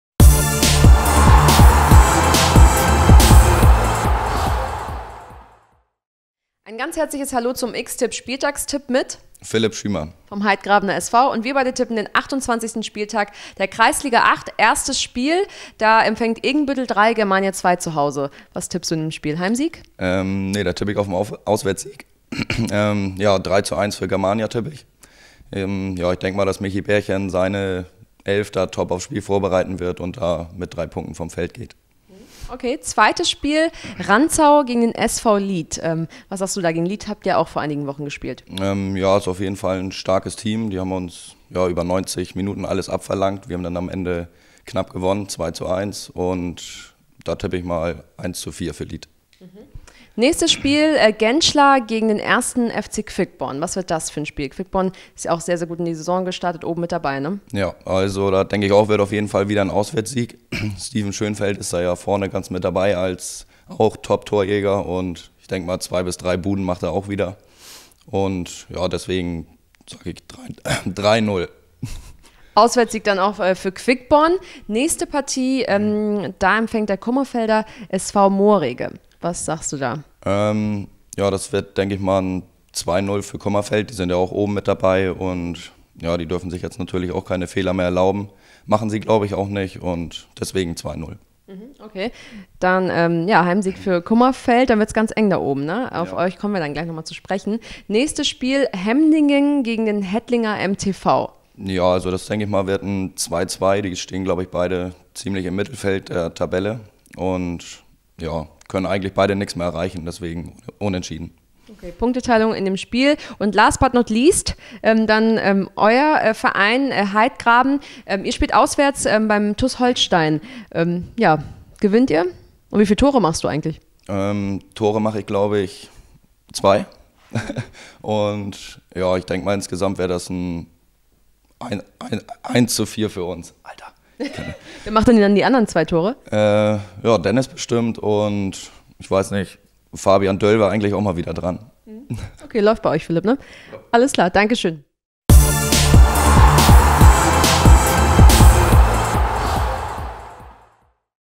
XTiP Spieltagstipp mit Philippe Schümann (Heidgrabener SV) - 28. Spieltag, Kreisliga 8 | ELBKICK.TV
Philippe Schümann (Heidgrabener SV) tippt den 28. Spieltag der Kreisliga 8. Stimmt Ihr zu oder liegt er daneben?

ELBKICK.TV und XTiP suchen Hamburgs Tippkönig! Dem Gewinner winkt am Ende der Saison eine attraktive Prämie.